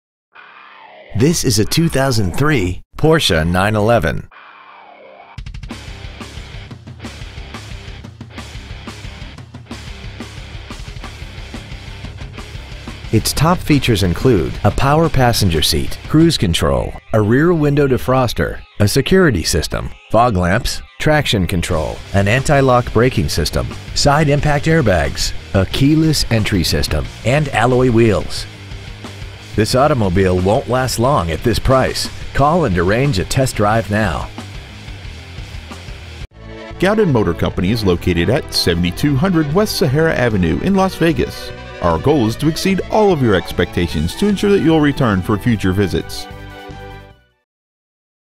2003 Porsche 911 Las Vegas NV
Year : 2003
 Make : Porsche
 Model : 911
 Engine : 3.6L H6 SFI DOHC
 Trans . : TipTronic
 Exterior : Carrara White

 Interior : Black
 Stock : J6986A

 Pre-Owned 2003 Porsche 911 Las Vegas NV